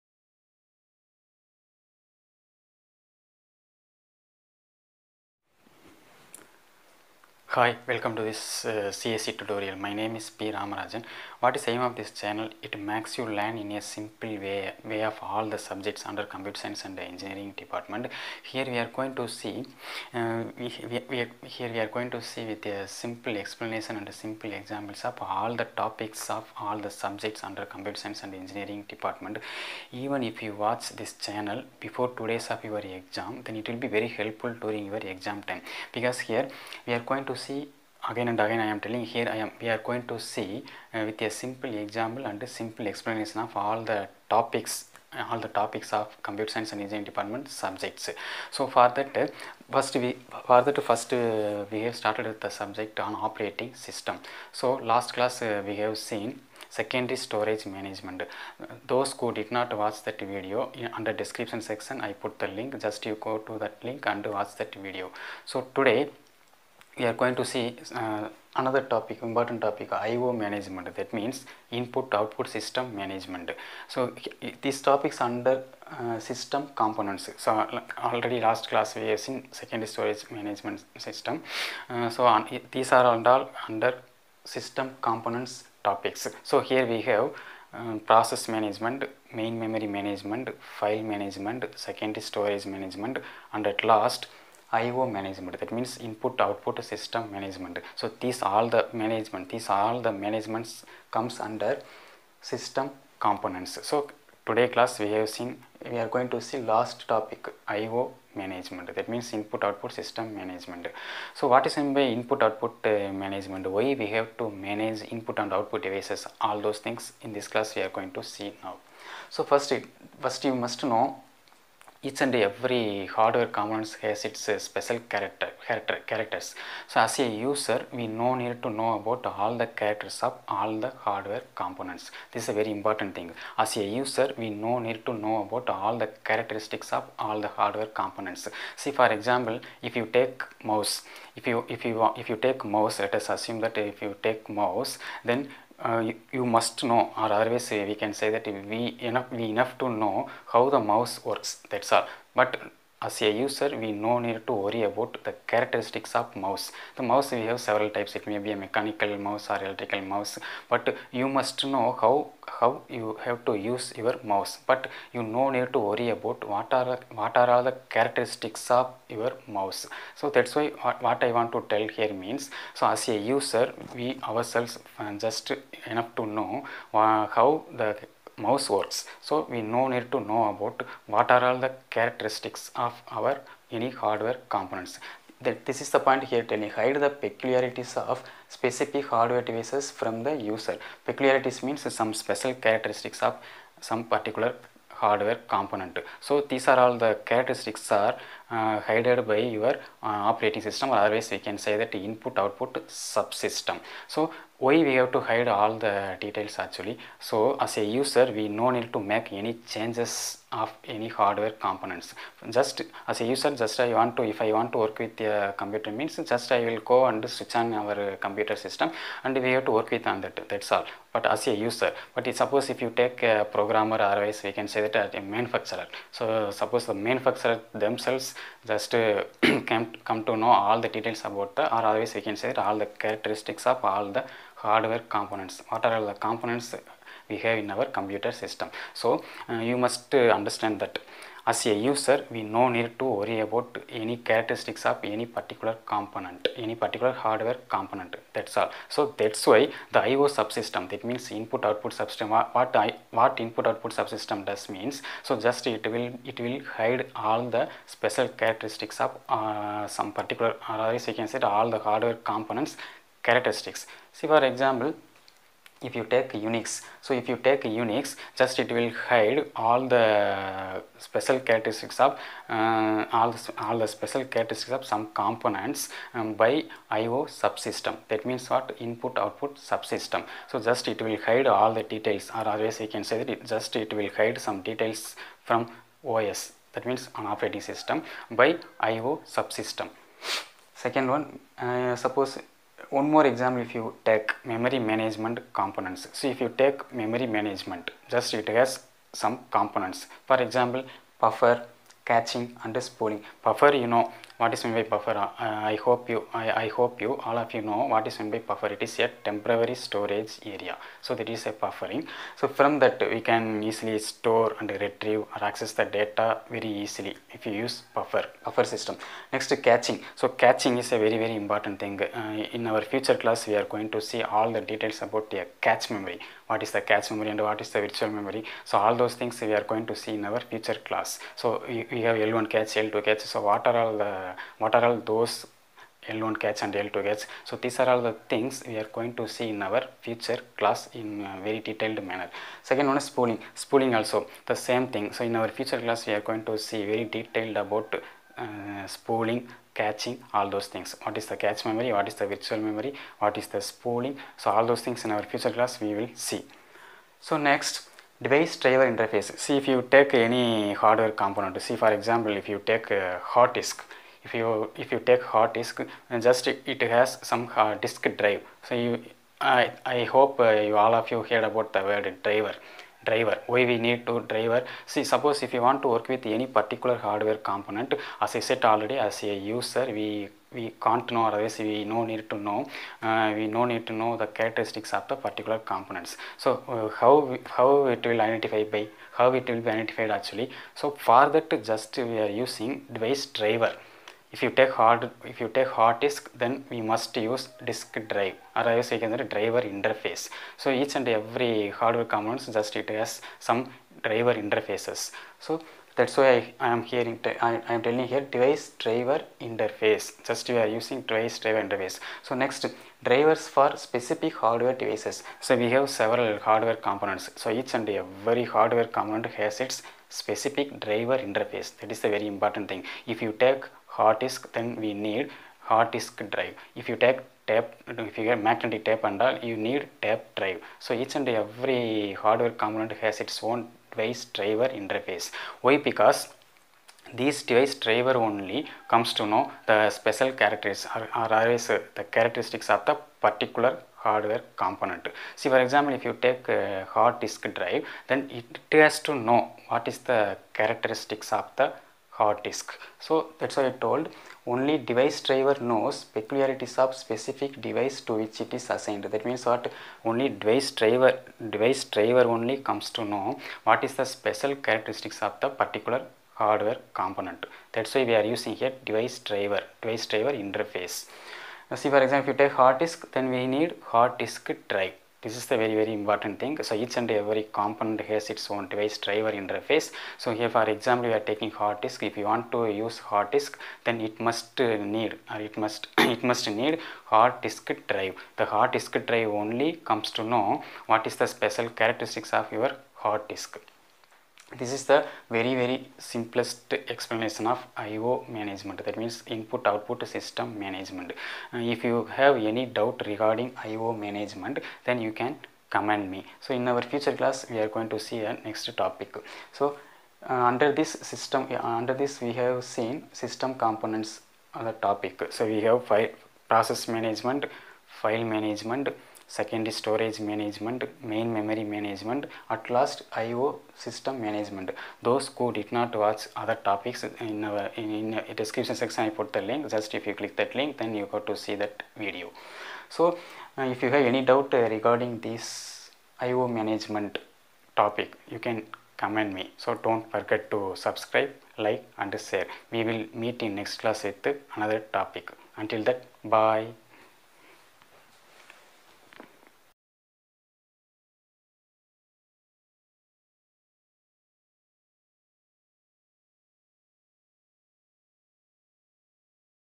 Input output management(simplified)in operating system
Hide the characteristics of hardware components by IO subsystem

Memory management components includes buffering caching and spooling

Needs device driver interfaces

Drivers for specific hardware devices

Watch previous video by clicking below link